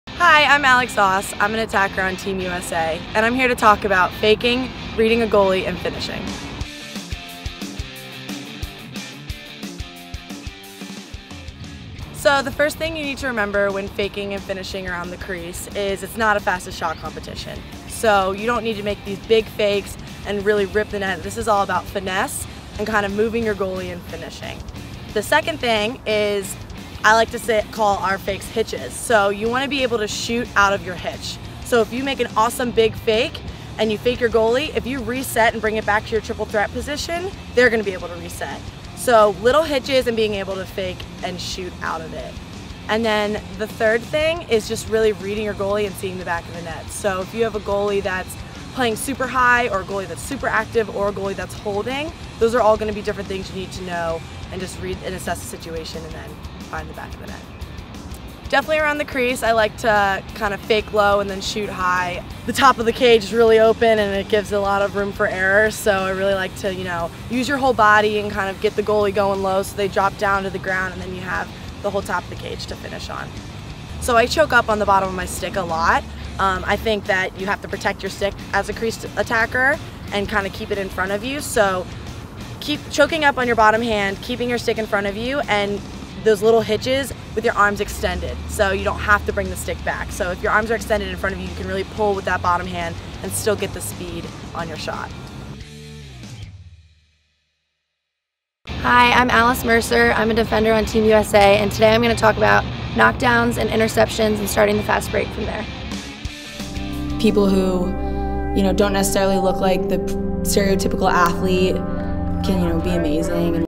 Your Edge: Faking and Finishing with Team USA's Alex Aust
Former University of Maryland Tewaaraton finalist and current Team USA attacker shares her tips for faking and finishing around the crease.

Lacrosse Magazine's "Your Edge" works with the best players in the game to reveal their secrets to success on the field.